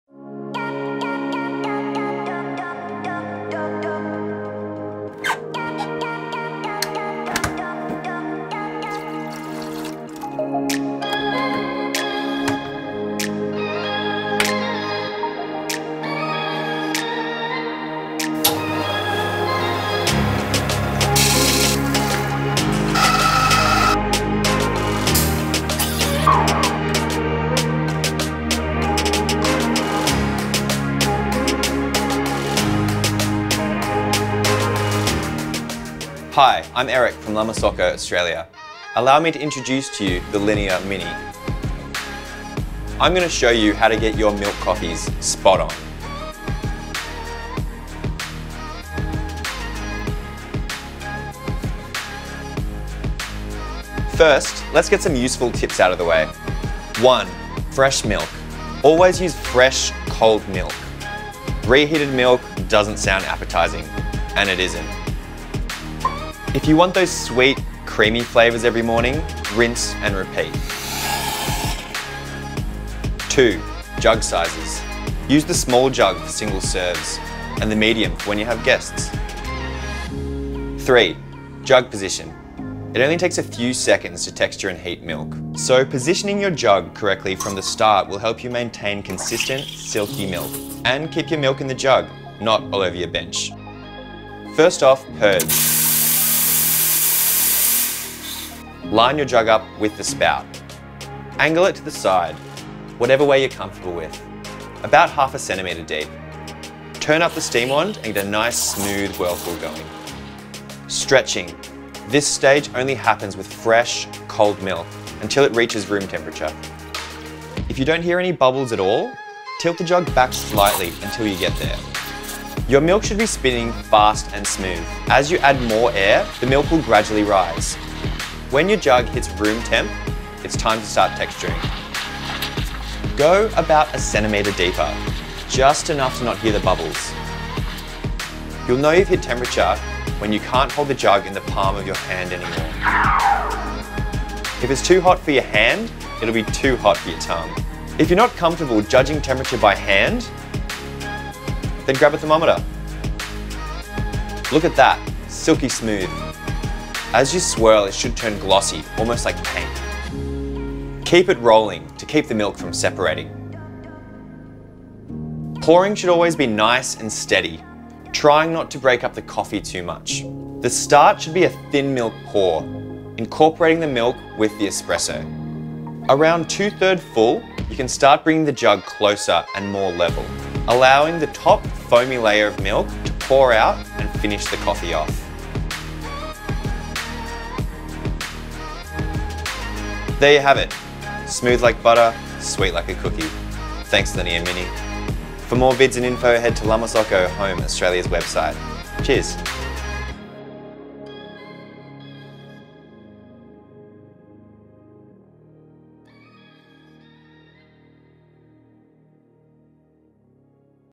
Milk steaming and Pouring
Finishing off the mini-series with La Marzocco Australia by
learning how to create great frothy milk using the Linea Mini.